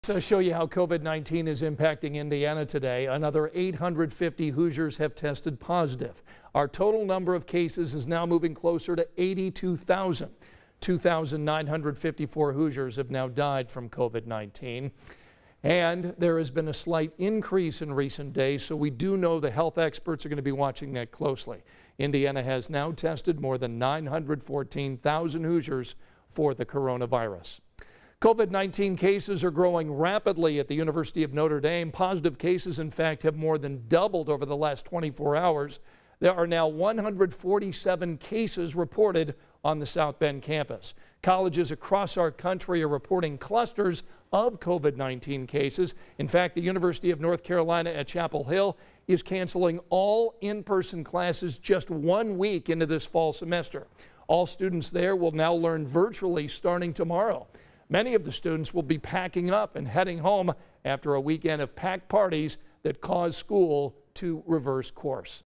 Some universities have changed course, sending students home after a spike in COVID-19 cases.
Here's the latest information available on COVID-19 in Indiana.